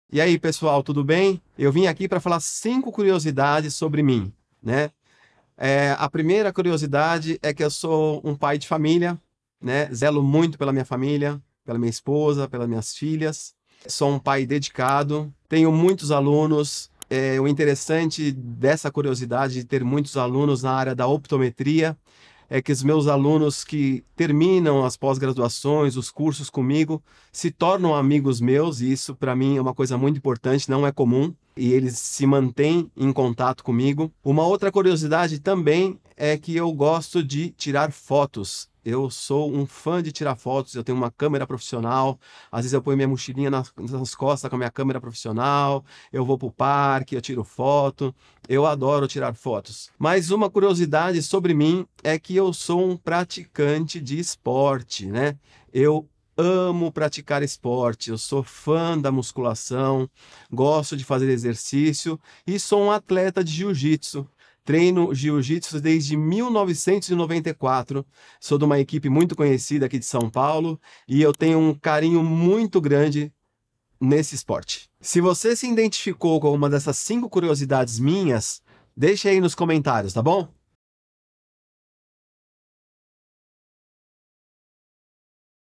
CURIOSIDADES SOBRE MIM | Sergey Cusato Jr - Optometrista
Dividir sobre minha vida pessoal e profissional é um prazer para inspirar meus pacientes e profissionais do ramo da optemetria.